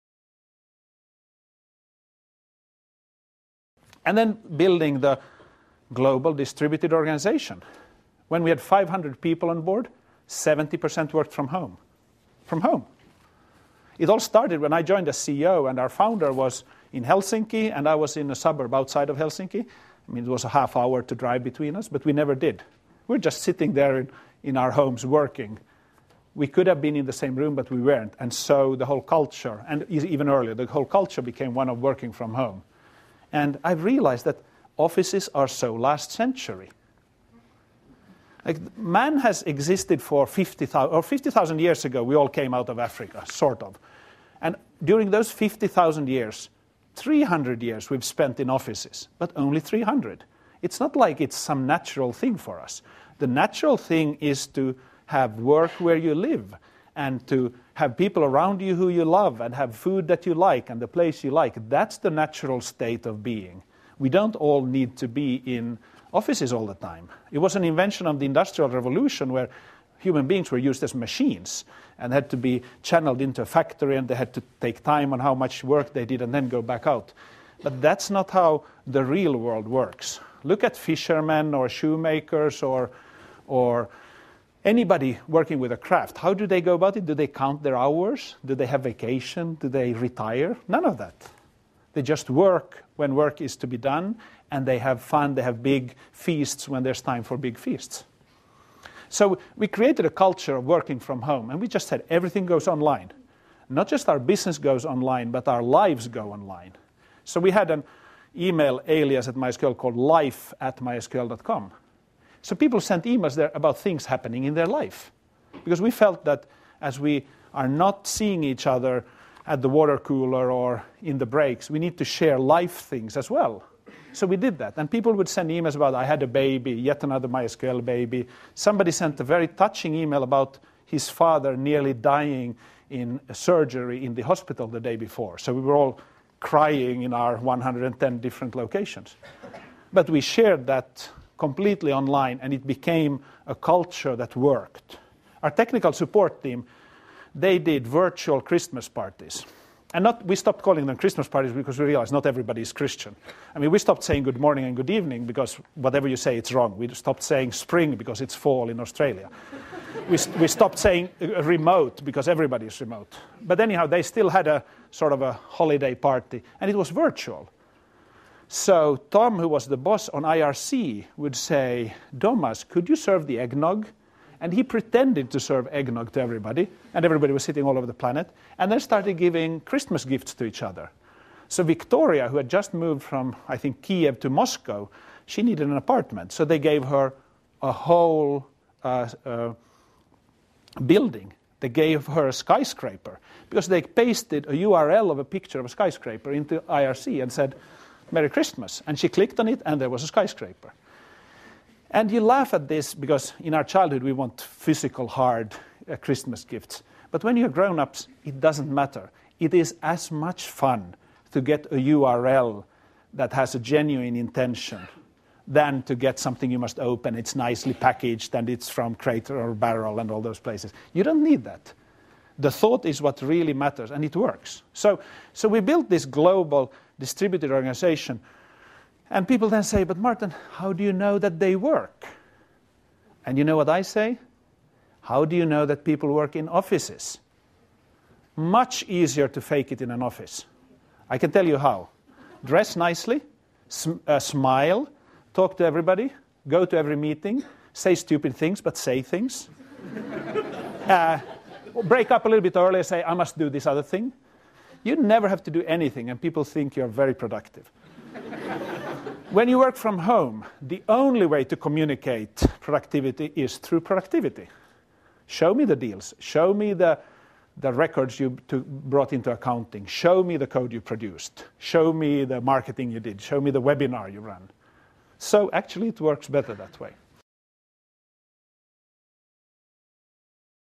Marten Mickos-Benefits of a Distributed Organization
As CEO of MySQL AB, Mårten Mickos saw advantages of utilizing a distributed organization model where seventy percent of the company's employees worked from home. According to Mickos, this cultural design actually put the focus on results and product